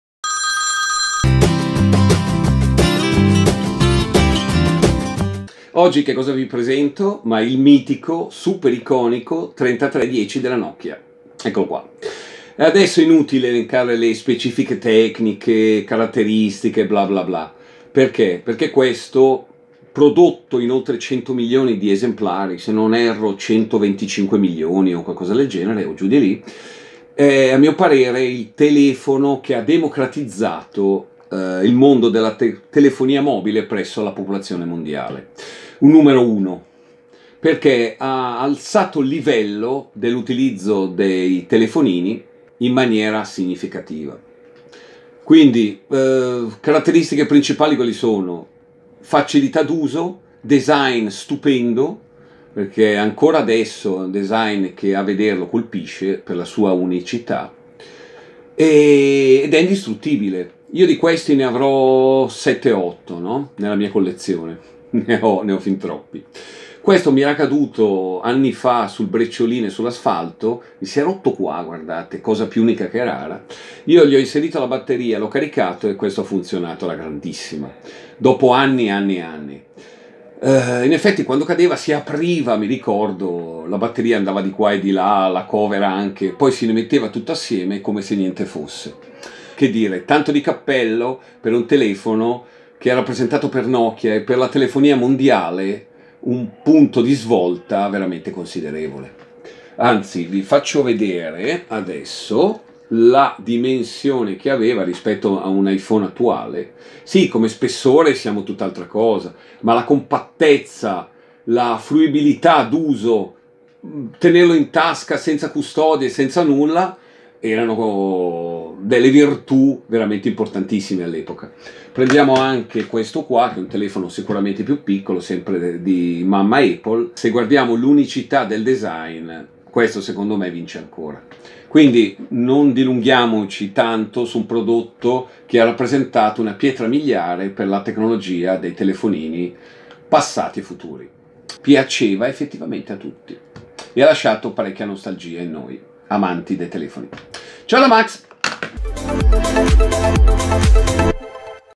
Telefono 51: nokia 3310 il telefono che ha democratizzato la telefonia!
Ciao da Max e dal canale smartmaxy.
Poche parole, video breve oggi, perchè? perché parlare del nokia 3310 non serve, basta osservarlo, basta tenerlo in mano per capire che cosa ha rappresentato per la telefonia mondiale. Ha democratizzato la telefonia sia nei paesi ricchi che quelli più a basso reddito, con una costruzione indistruttibile ( può essere tranquillamente acceso e utilizzato or ora e funziona! ndr), e modalità semplici ma solide che servivano egregiamente sia al businessman che al giovane o al padre di famiglia. Insomma tutto si sa di questo telefono, non sono le immagini o i video a parlare, ma è la storia che fa di questo modello , prodotto in più di 120mln di esemplari, un fuoriclasse.
IL TELEFONO PRECISO, FA PARTE DELLA MIA COLLEZIONE PRIVATA.
Ora via al video e grazie ancora per aver visitato il canale!
Alla prossima recensione!